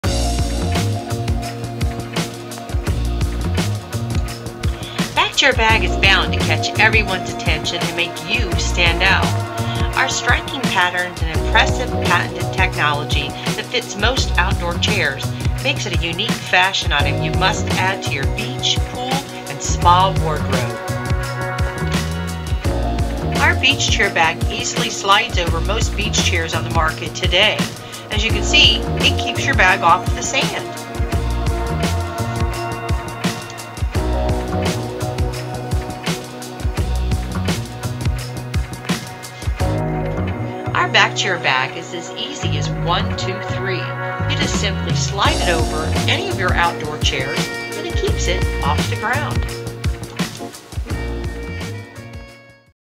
2019 Back Chair Bag video
"We keep your beach & pool essentials dry and sand free!"

The first functional Outdoor Back Chair Bag with our Innovative
patented BCB Technology design with a easy ON & OFF sleeve design
that fits on the back of most outdoor chairs.

Back Chair Bag
Winter Park, Florida 32792